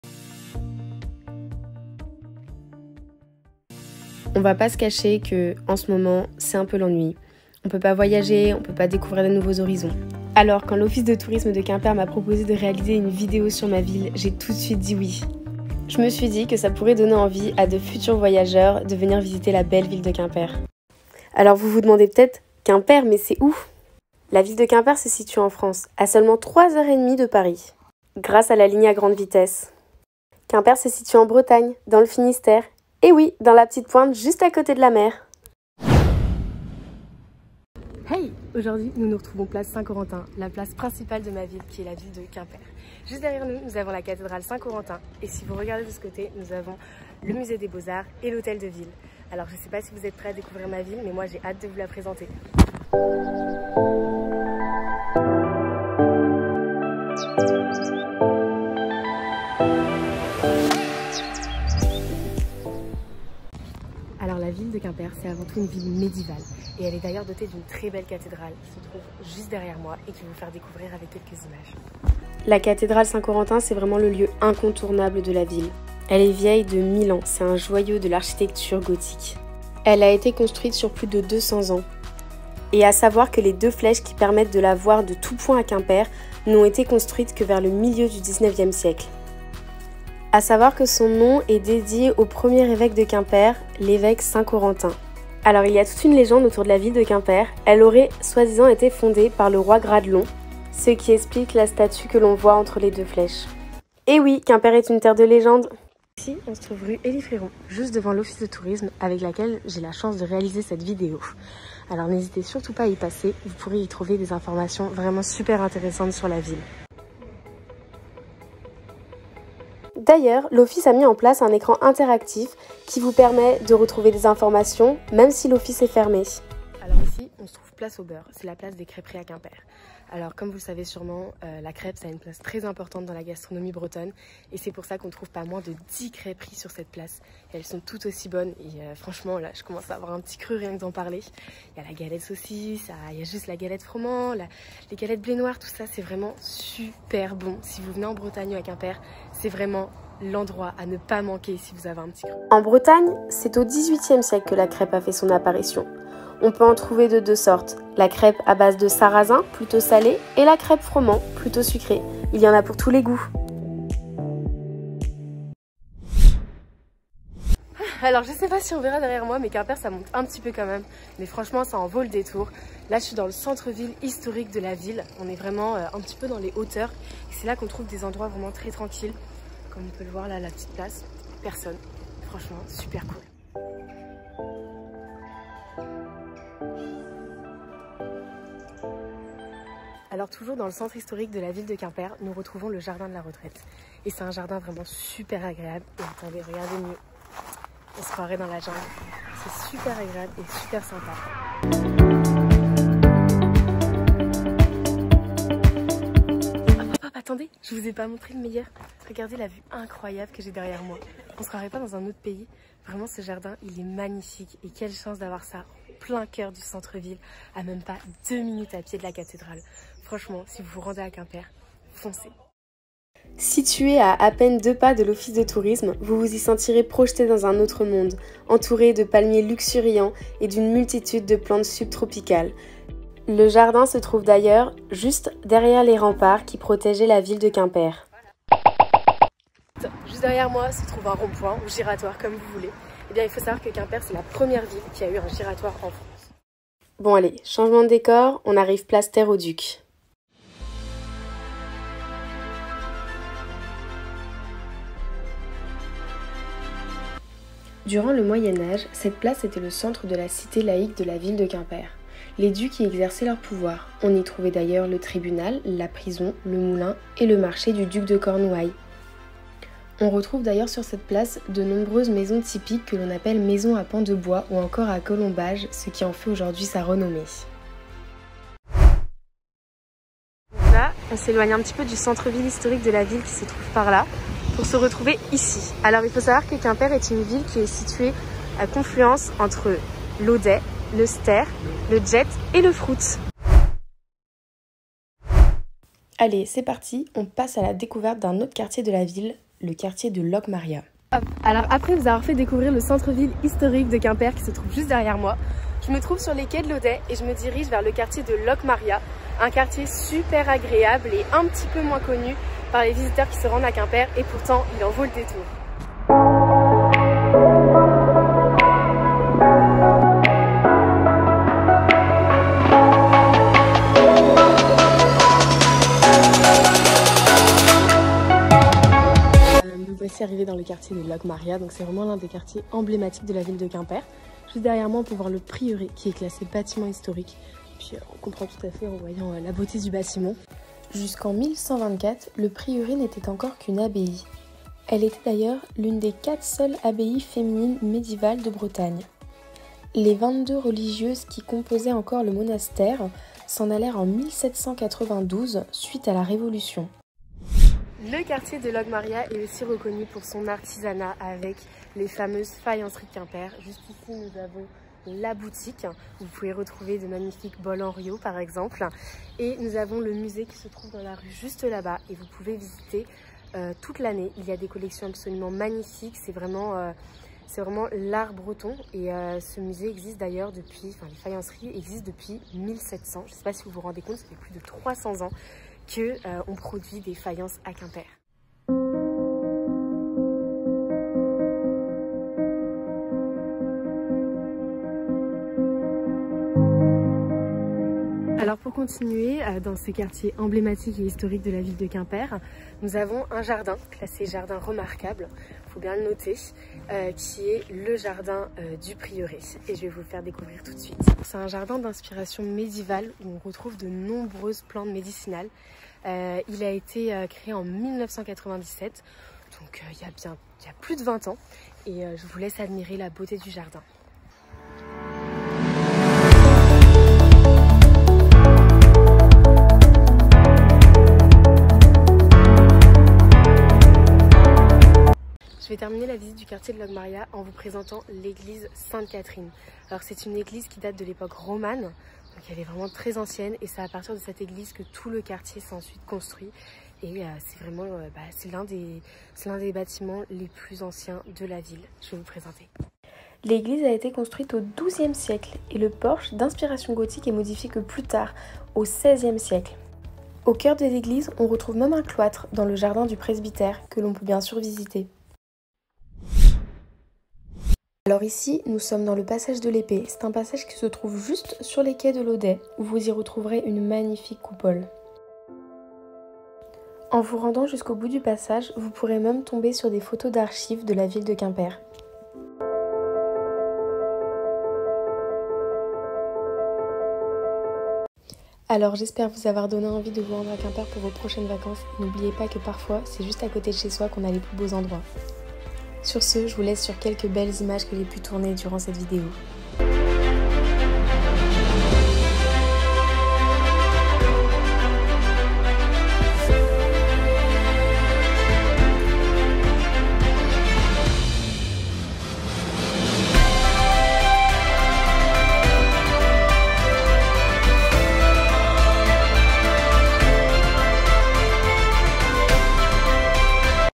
QUIMPER, UNE VILLE ULTRA AGRÉABLE !
Aujourd'hui je vous présente ma ville : Quimper !
J'espère que ça va vous donner envie de vous rendre en Cornouaille pour vos prochaine vacances !

N'hésitez pas à commenter votre ville dans les commentaires en me donnant envie de m'y rendre !

Rendez-vous sur l'application gratuite "CIRKWI" ou sur le site internet pour découvrir le circuit de ma visite et plus d'informations

Logiciel utilisé pour le montage :
Vidéolap PRO

Merci pour votre visionnage et à bientôt pour de nouvelles aventures !